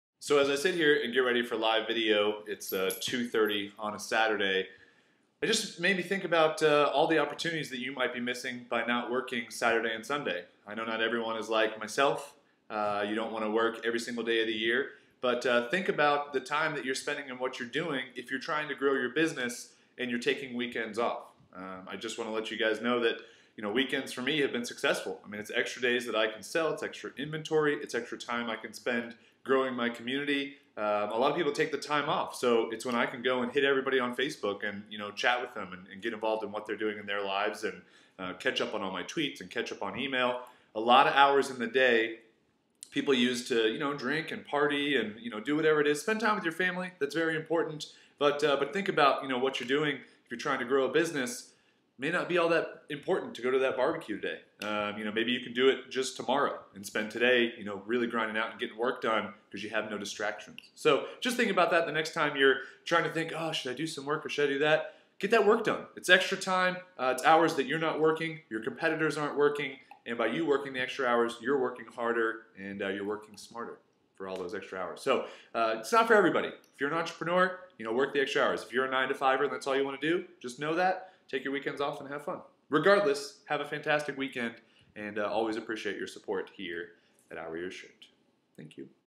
I worked 400+ more hours than most people last year, you?
I firmly believe the extra 104 days I worked last year helped build iwearyourshirt.com. If I only worked 4 hours a day on the weekends, that's still 400 more hours than my competition. That adds up to TEN 40-hour work weeks. That's 400 more hours (or 10 more weeks) I could interact on Facebook, Twitter, etc. That's 400 more hours of learning, experimenting and building my business. Was it worth missing a few barbecues with friends and parties here and there? 100% because I'm trying to build a business.